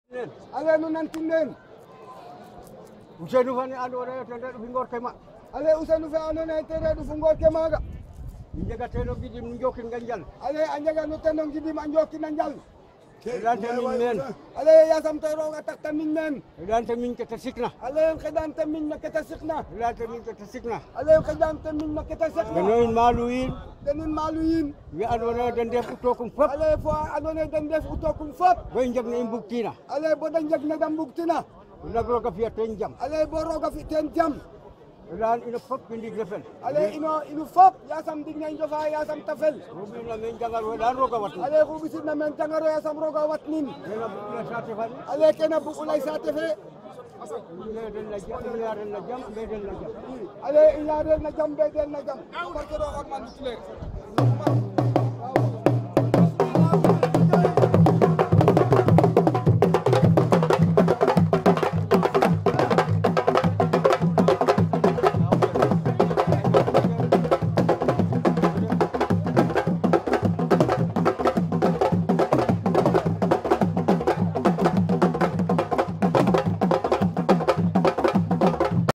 kouma Assane Bouda Dogue s'adresse à la population
Le kouma (chef spirituel) Assane Bouda Dogue s'adresse à la population à la fin du bois sacré de Mar-Lothie de 2017